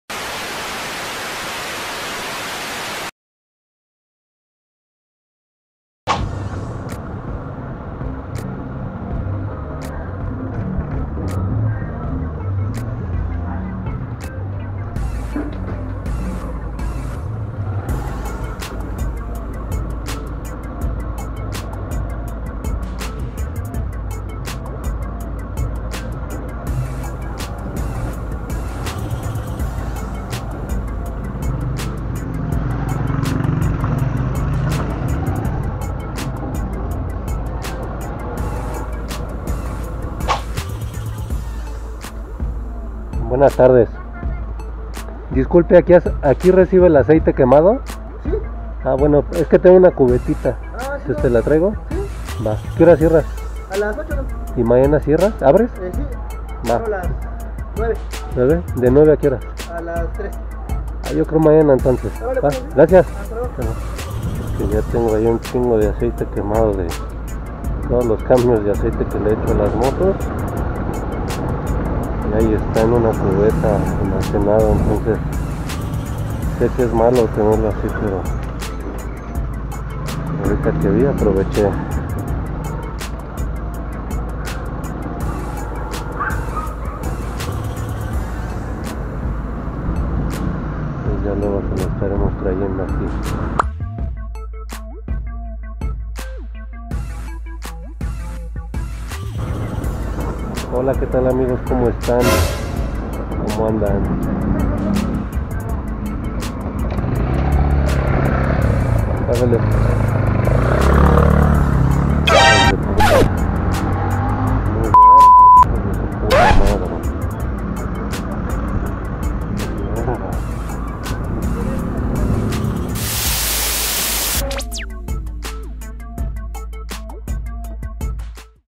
Ruleteando Un Rato y Me Encuentro Con Un Moto taxista Vale Madres 💪 (Año 2017) / NORMAN
Nunca faltara un p3nd3jas0 de estos, que estén muy bien

Si puedes regalarme un like y compartir estaría súper 🙏

Compartiendo mis experiencias

gitup git2 pro

Instagram, Tik tok, Facebook: Norman Moto